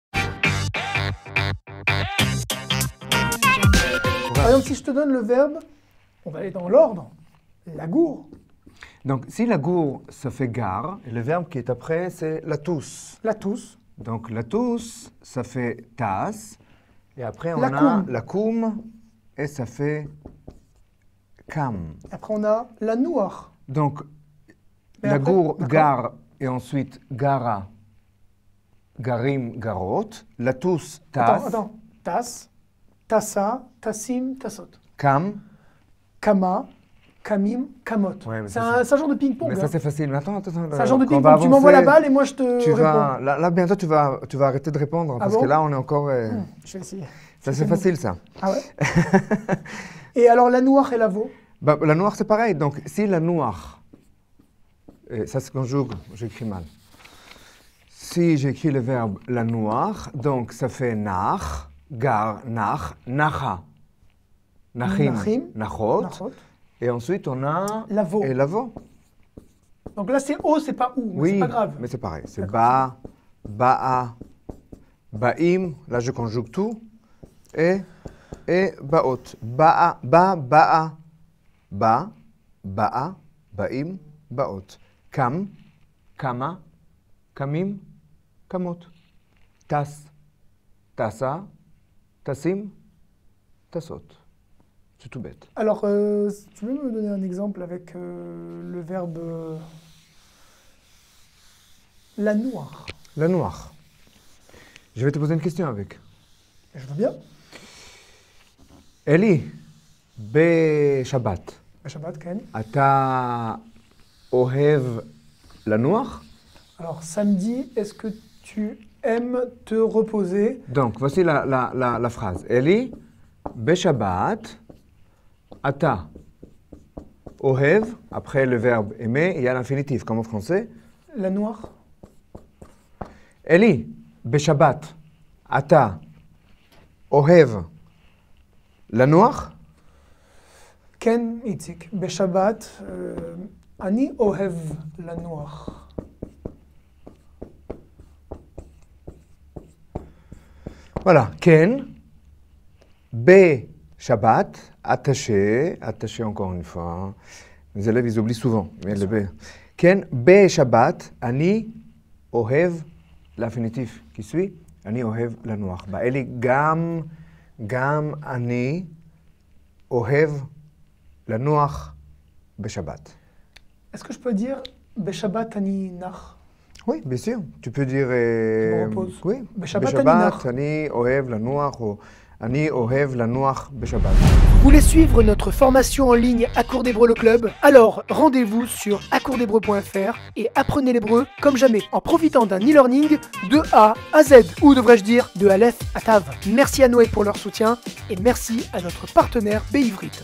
Leçon 12 Débutants - Récap verbes ( extrait ) - Formez-vous sur
Savez-vous que nous avons appris jusqu'à maintenant près de 25 verbes ? Ça avance ! Nous voudrions dans ce cours nous arrêtez un instant pour faire un point sur la conjugaison hébraïque au présent et opérer quelques rapprochements entre les différents verbe afin de vous montrer les similitudes de comportement entre eux. Alors si vous le voulez bien mettons de l'ordre dans les esprits.